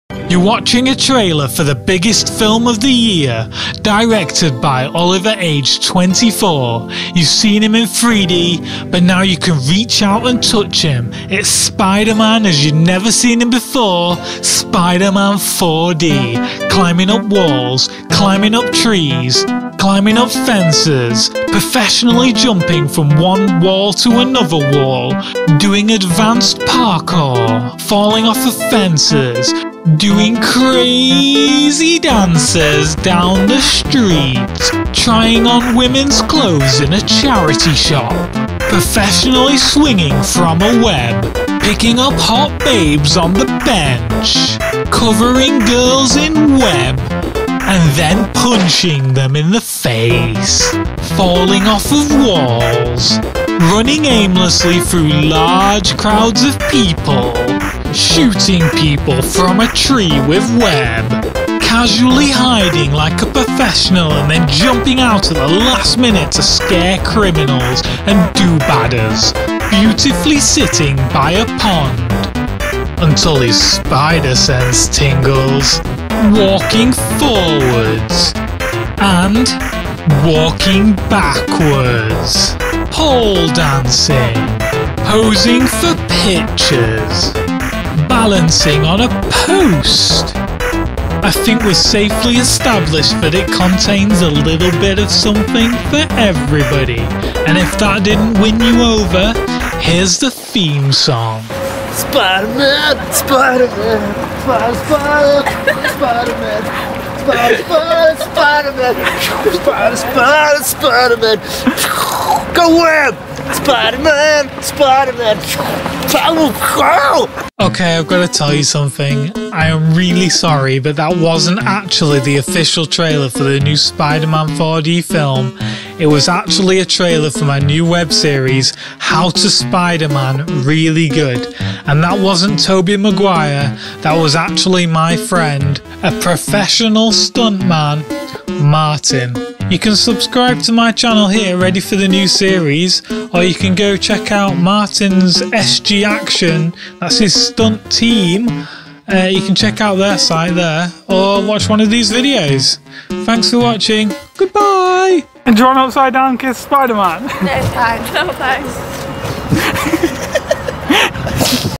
Spiderman 4D - Official 3D Trailer 2013
The Official 2013 Trailer for Spiderman 4D.

Directed, Filmed, Edited and Narrated by Oliver Age 24.

Starring Martin Wan as Spiderman - Performing and choreographing his own stunts.